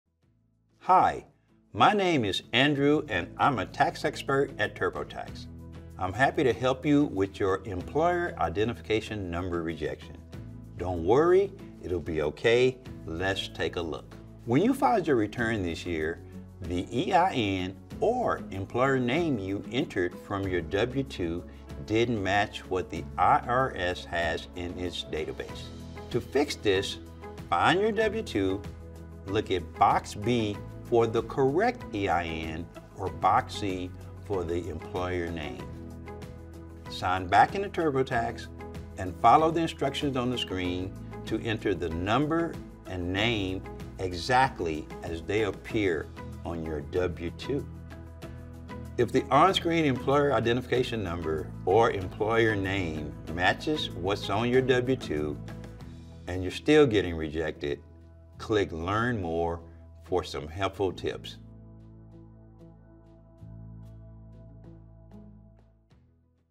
How to fix an EIN mismatch e-file reject - TurboTax Support Video
Learn how to fix your EIN mismatch e-file reject FW2-502. for complete instructions and tips.